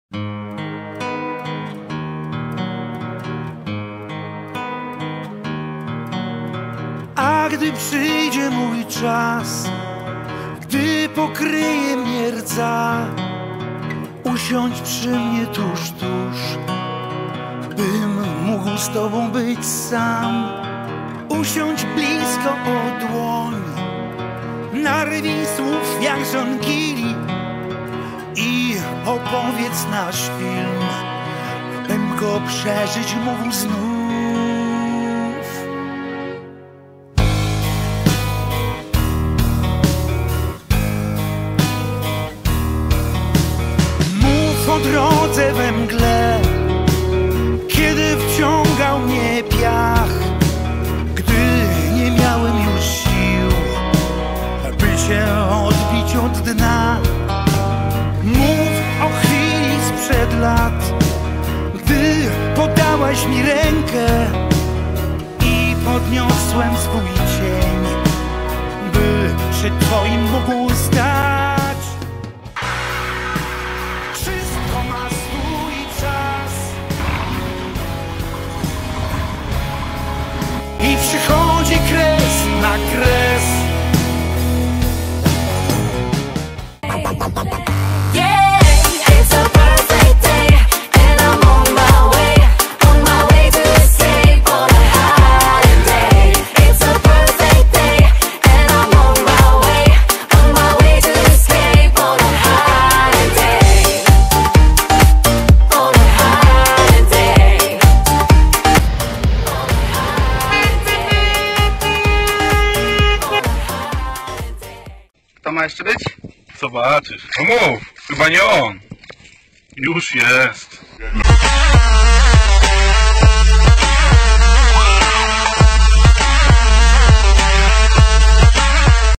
Rotor is not death czyli NOWE NABYTKI 2024
Witajcie!
W tym roku przyszedł czas na nowe nabytki :)
Uprzedzając pytania Ursus zostaje :)